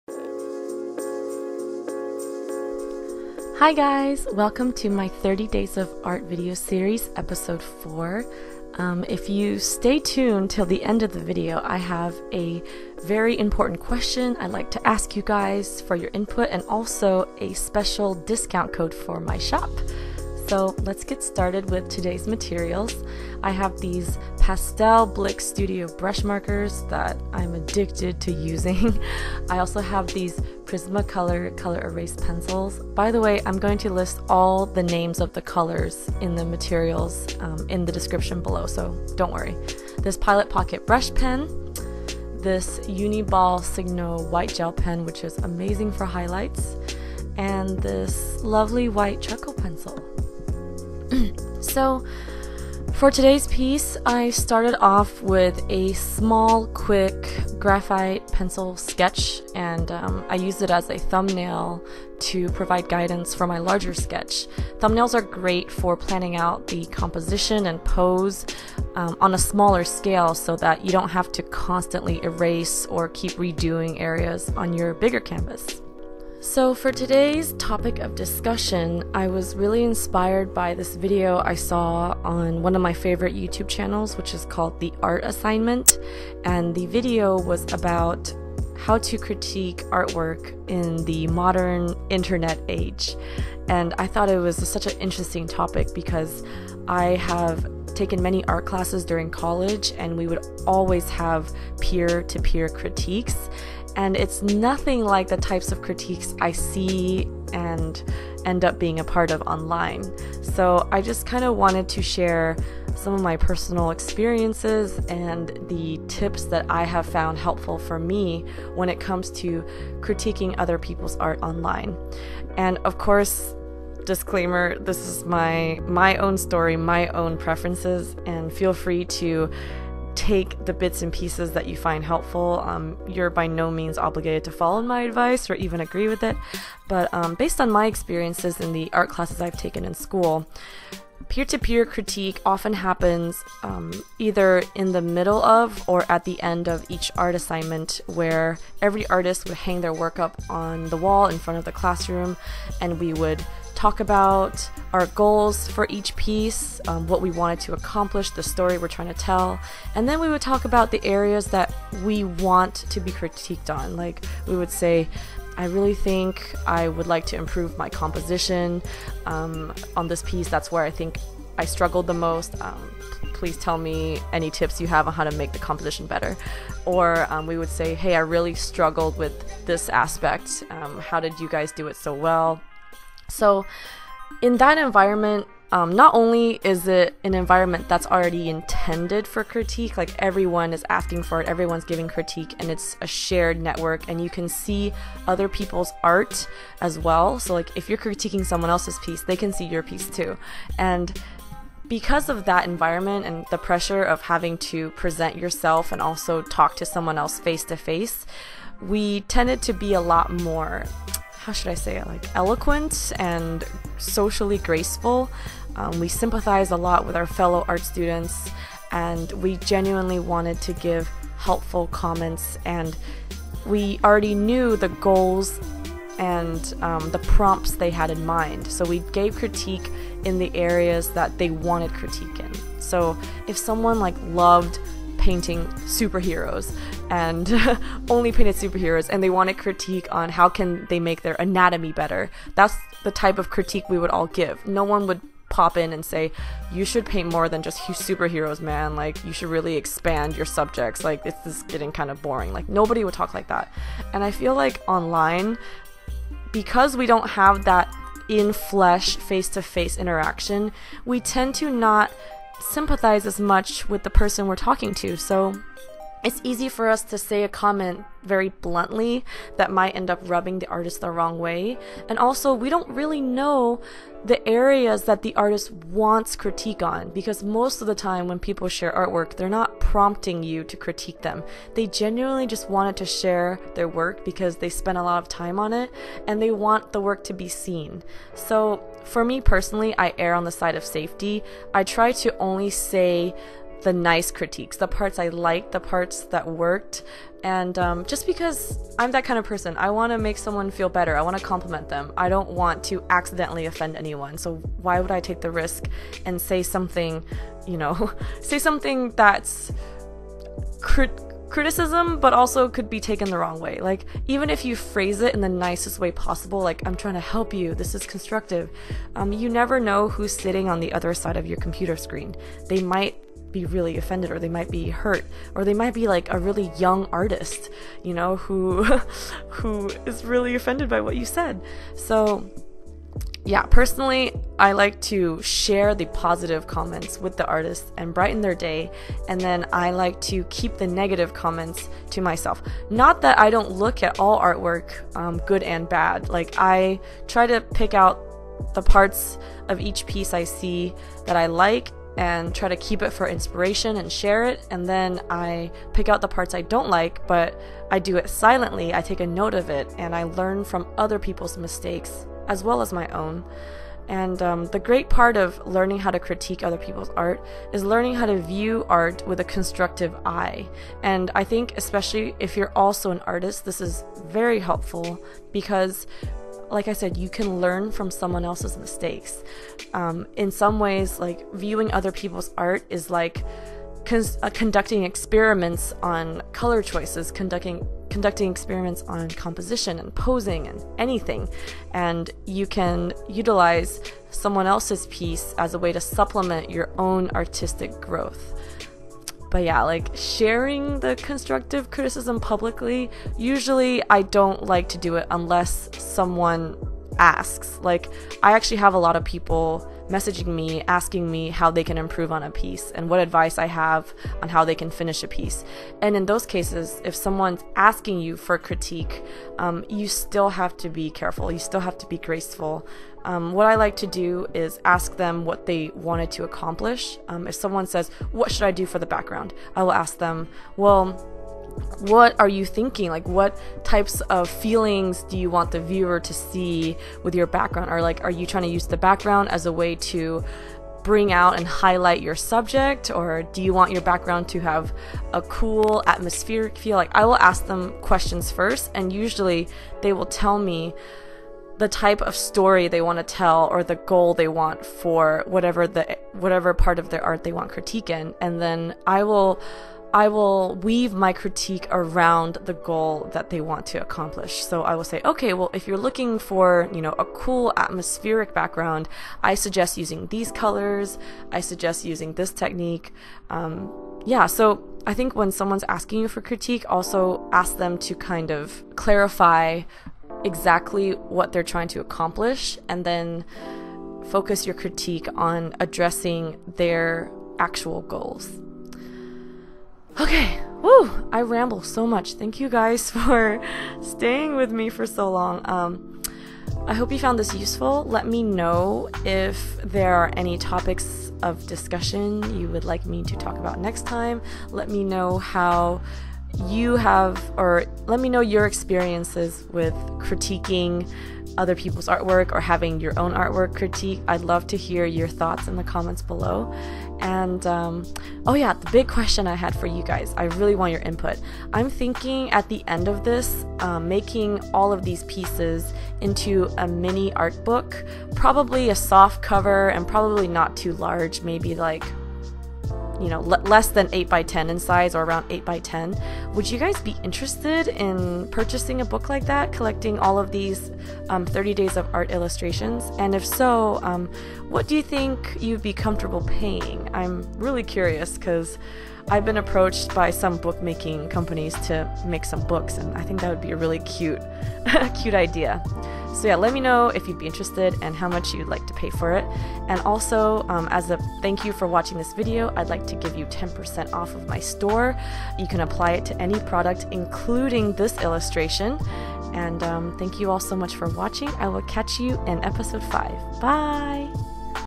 Judging other people's art || 30 Days of Art Episode 4
Hi there! Today I will be drawing a mermaid with colored pencils and markers and discussing how to critique other people's art.

Happy

Materials I used in this video:
♥ Blick Studio Brush Markers: Warm Gray 10%, Rose Petal, Shell, Beach, Horizon Blue, Antique White, Lt. Cerulean Blue, Light Periwinkle
♥ Copic Ciao Marker: Pale Green
♥ Uniball Signoball Gel Pen: White
♥ General's Charcoal Pencil: White
♥ Pilot Pocket Brush Pen: Black
♥ Prismacolor Col-Erase Pencils: Carmine Red, Brown, Terra Cotta, Light Green, Pink, Light Blue, Canary Yellow, Rose, Light Peach, Black, Indigo Blue

A very SPECIAL thank you to my lovely patrons, without whom this video would not have been possible. ♥♥♥